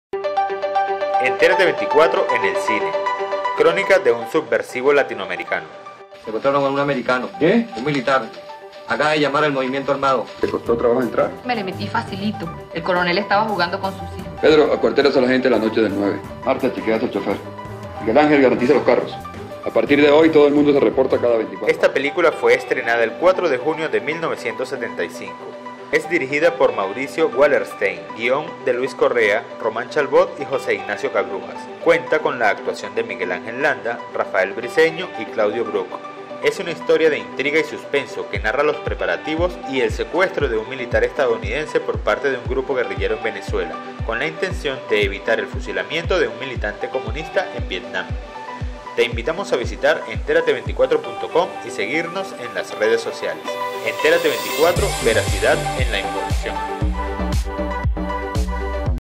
45 años del estreno de "Crónica de un subversivo Latinoamericano" - Película 4 de junio
- ENTÉRATE 24 EN EL CINE -

CRÓNICA DE UN SUBVERSIVO LATINOAMERICANO.

ESTA PELÍCULA FUE ESTRENADA EL 4 DE JUNIO DE 1975.

ES DIRIGIDA POR MAURICIO WALERSTEIN, GUIÓN DE LUIS CORREA, ROMÁN CHARBAUD Y JOSE IGNACIO CABRUJAS, CUENTA CON LA ACTUACIÓN DE MIGUEL ANGEL LANDA, RAFAEL BRICEÑO Y CLAUDIO BROOK.

ES UNA HISTORIA DE INTRIGA Y SUSPENSO QUE NARRA LOS PREPARATIVOS Y EL SECUESTRO DE UN MILITAR ESTADOUNIDENSE POR PARTE DE UN GRUPO GUERRILLERO EN VENEZUELA CON LA INTENCIÓN DE EVITAR EL FUSILAMIENTO DE UN MILITANTE COMUNISTA EN VIETNAM.

ENTÉRATE 24 VERACIDAD EN LA INFORMACIÓN.

Video realizado por Daniel Torrealba para Entérate 24